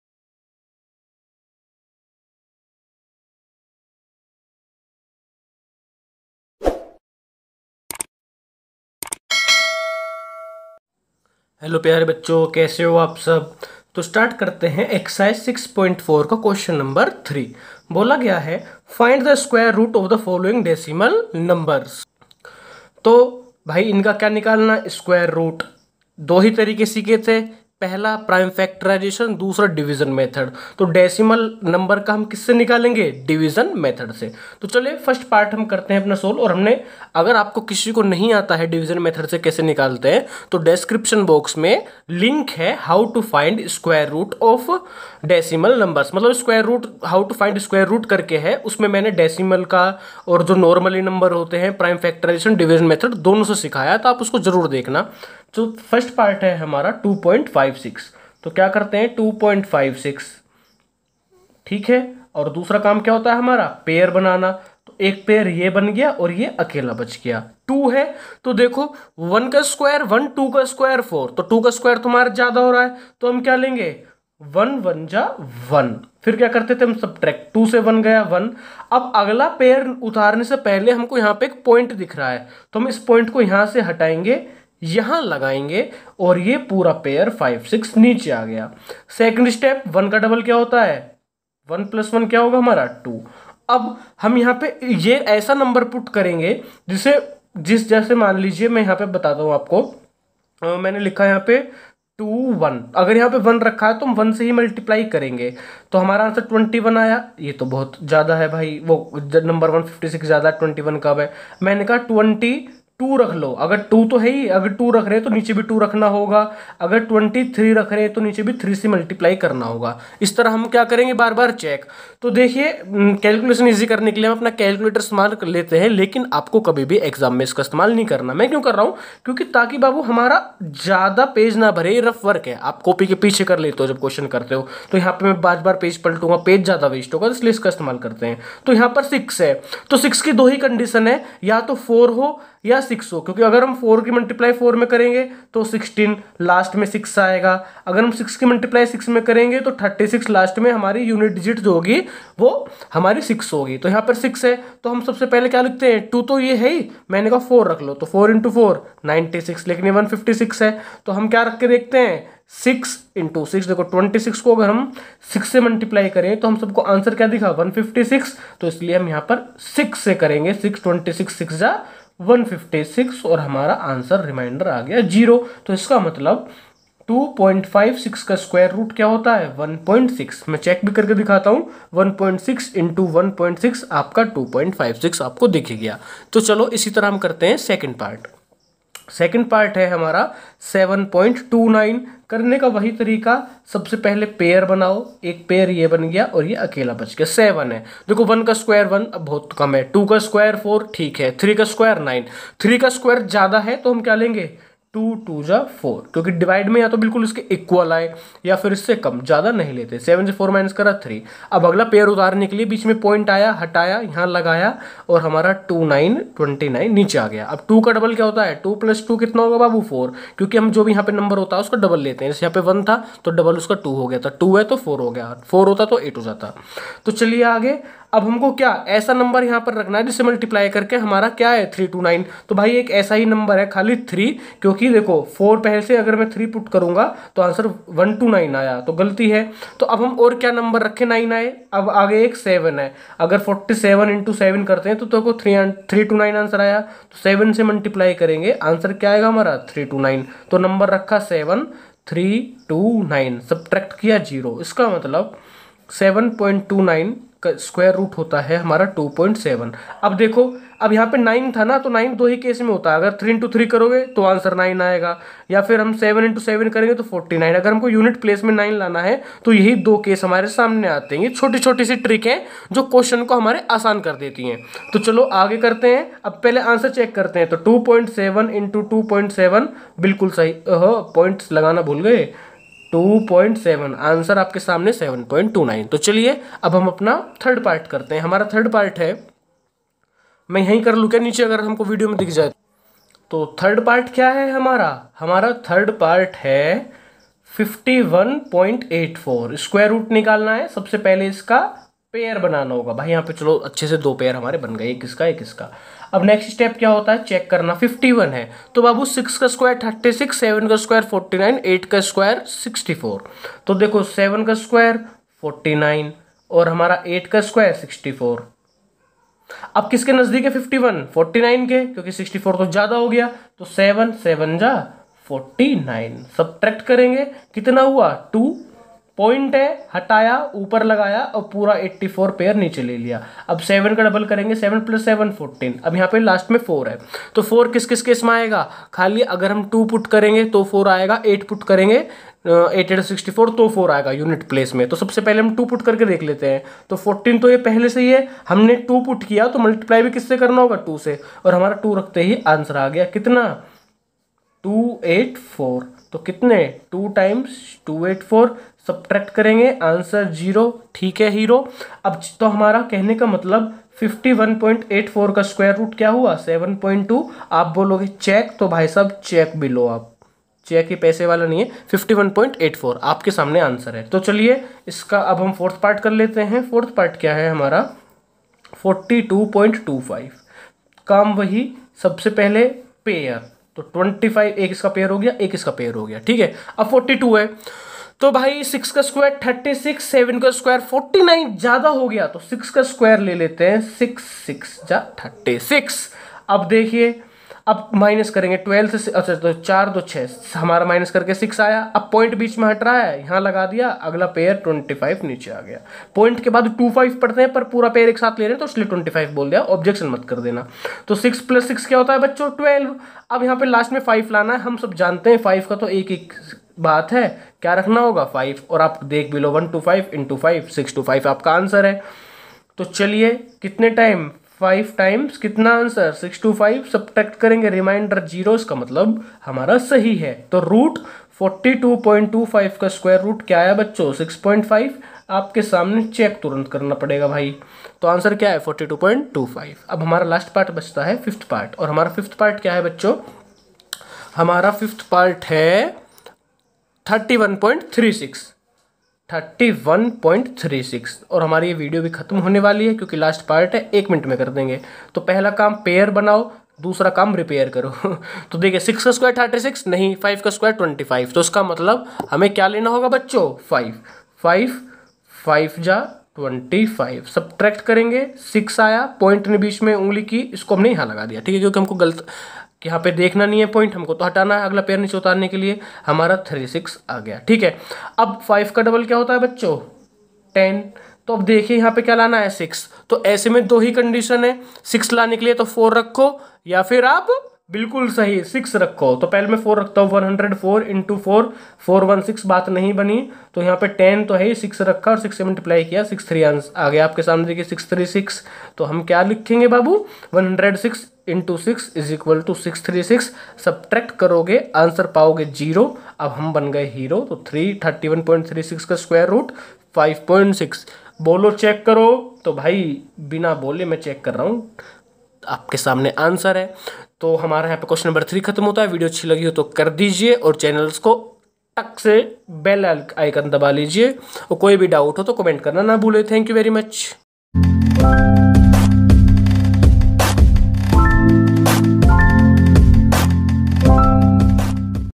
Q3 Ex6.4, Class 8th Math Ch-6 Squares And Square Roots
In this video we discussed Soluton of Q1 Ex6.4 of Ch-6 Squares and Square Roots .Do Comment if you have any doubt related to this video...
class8maths cbseclass8maths ncertclass8maths cbse2020-21#mathsclass8